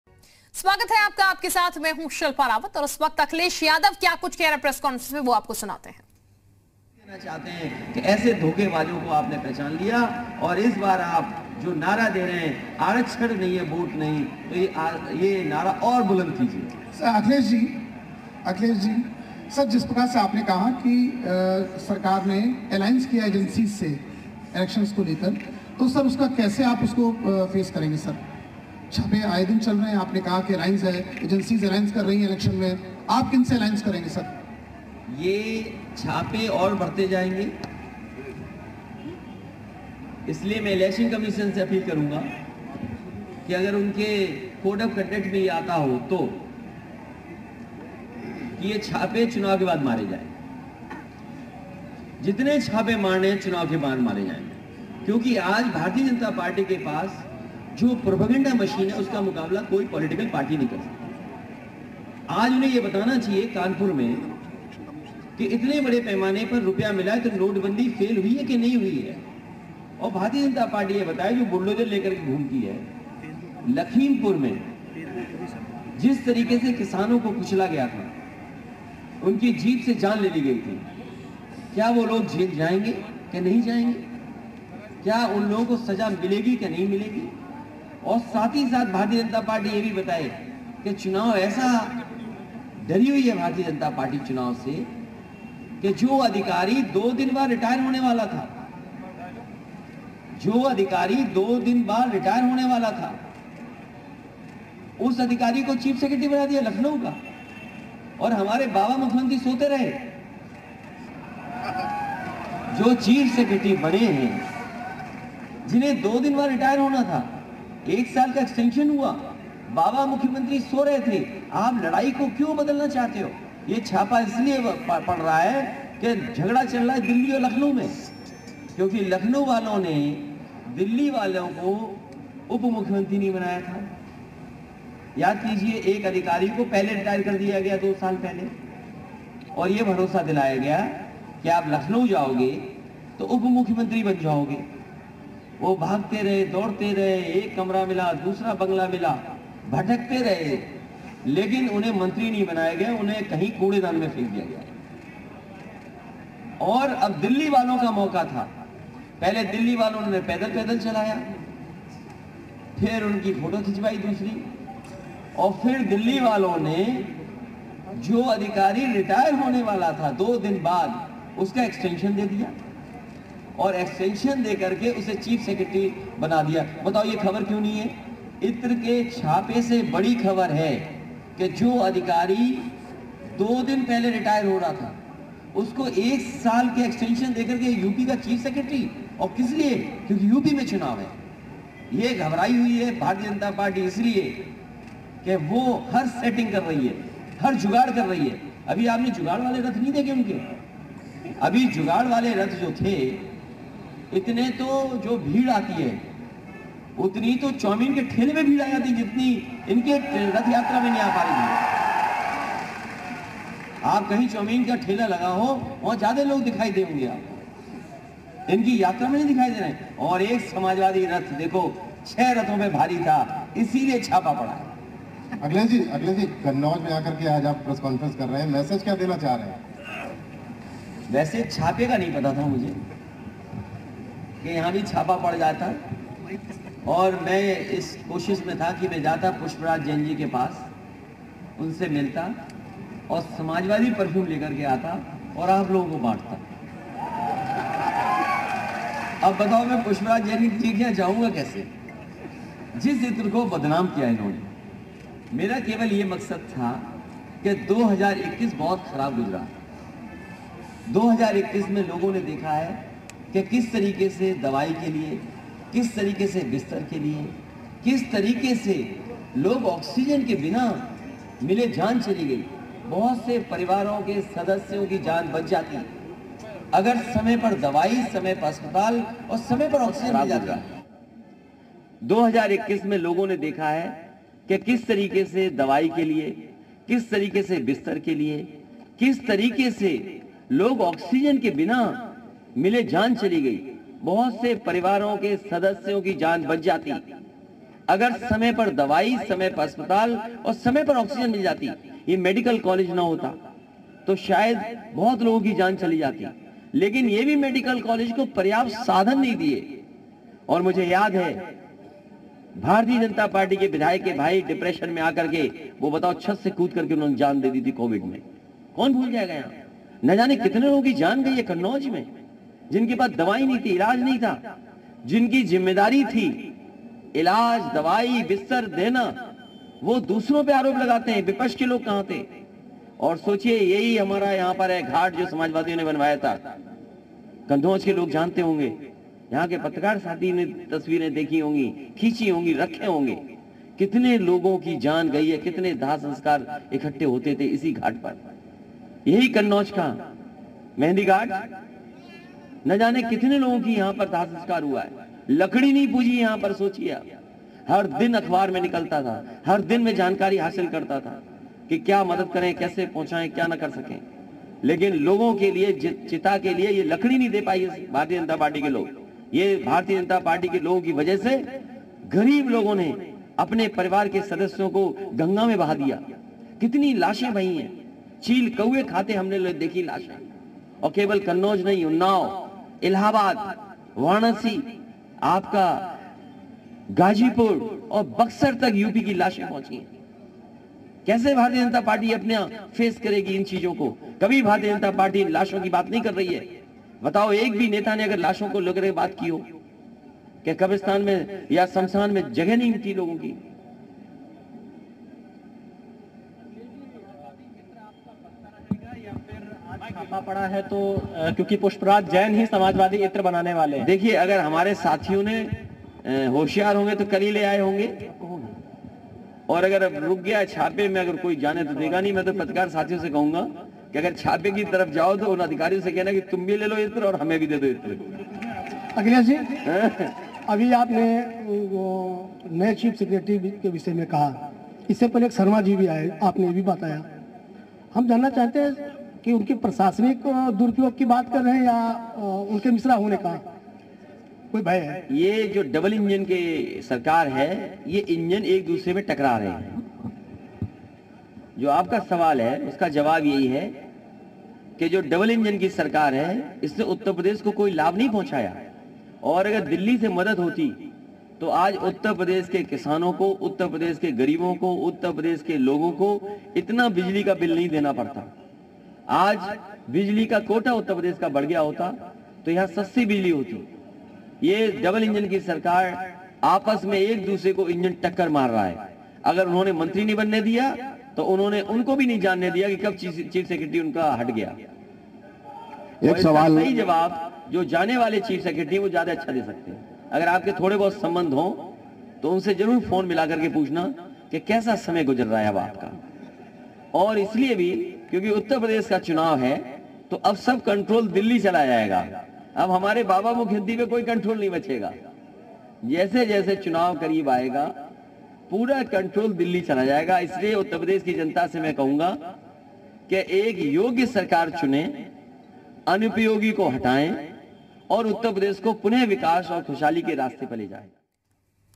अखिलेश यादव ने BJP पर साधा निशाना बोलें 'पीयूष जैन से BJP के हैं रिश्तें' | UPElection2022
प्रेस  कॉन्फ्रेंस में अखिलेश यादव ने bjp पर साधा निशाना बोलें 'पीयूष जैन से bjp के हैं रिश्ते'
About Channel: